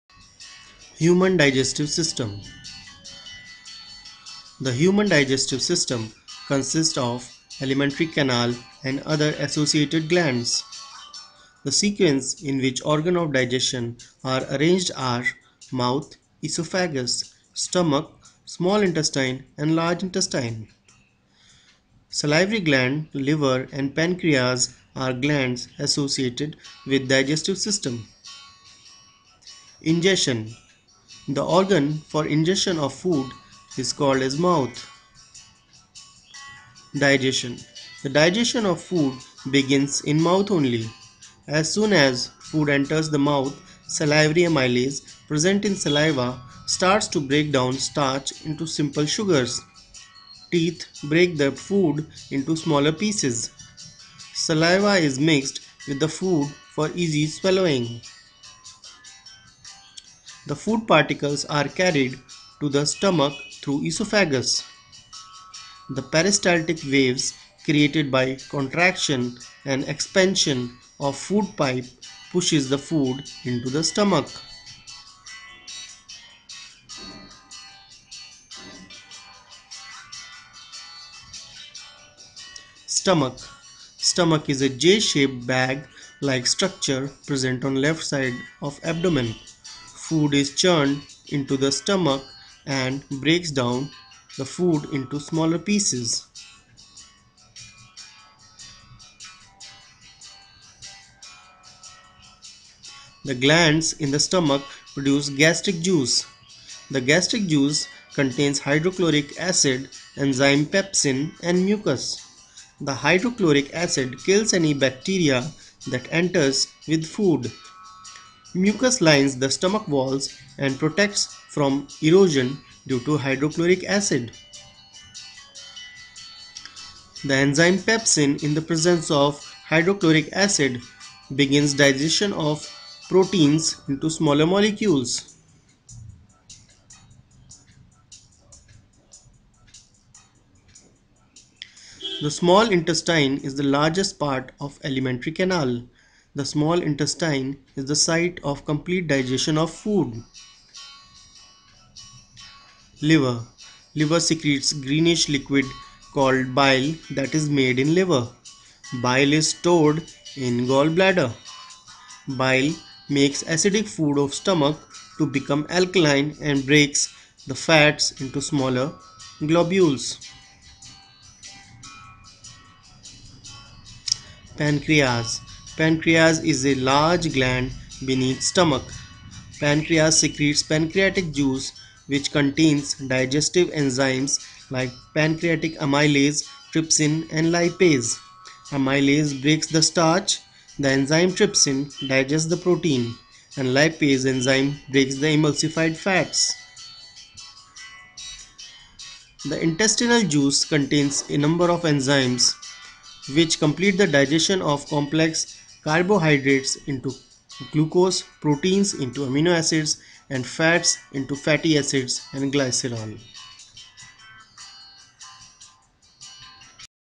Human Digestive System
Human digestive system comprises of mouth,esophagus,stomach and intestines.The food is broken down into smaller particles and absorbed by body.